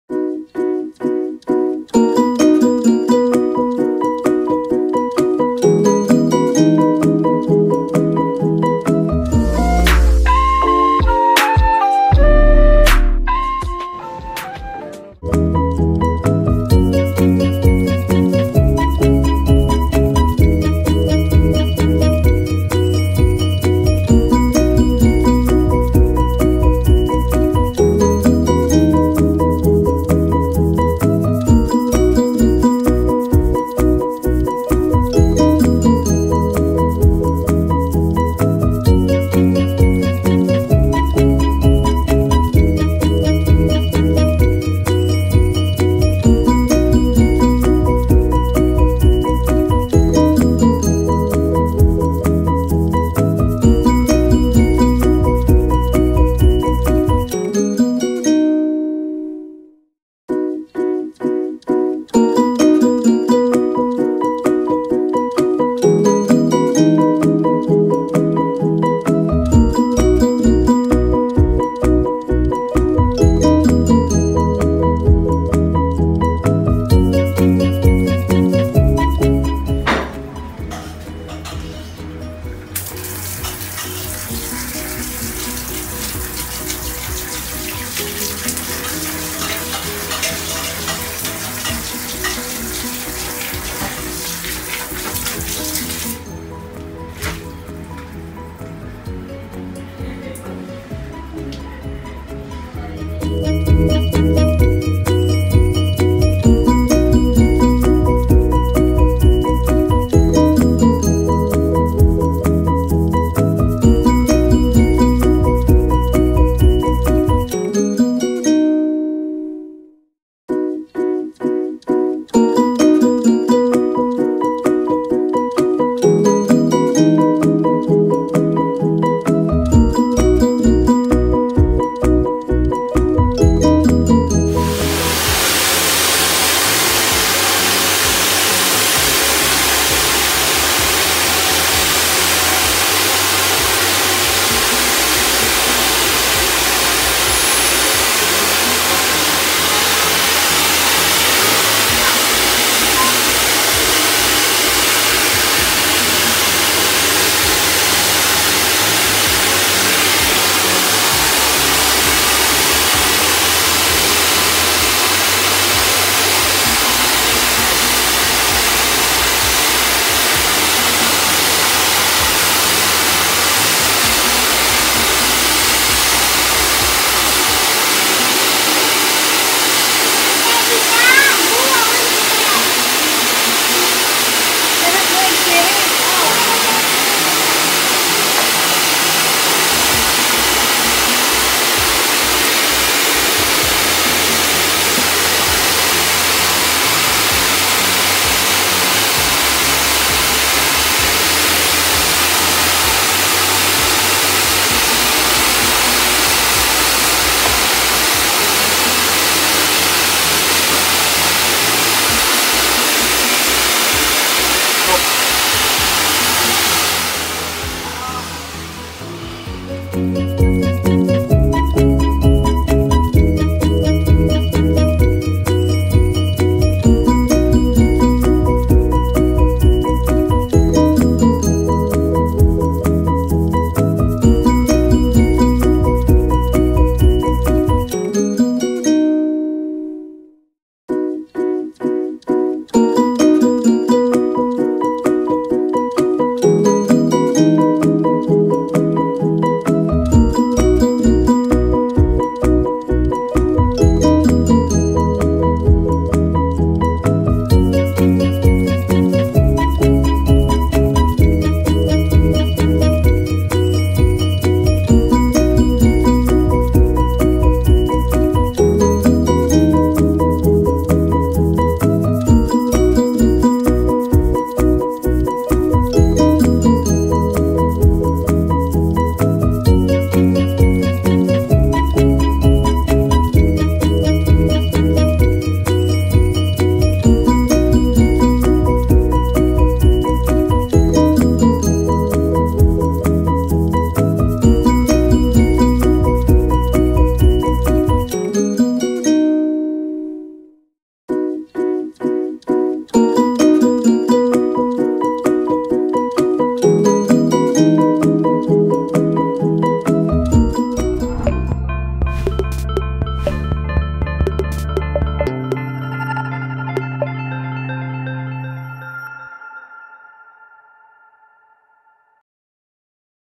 Aircon Maintenance | Vacuum Air Conditioner Unit | Wash Aircon Filters
If you want to keep your air conditioner in optimal condition for years, you simply cannot ignore to clean and check for its maintenance. Regular maintenance not only helps extend its life span, but improves its cooling efficiency and saves you big bucks on future aircon repairs.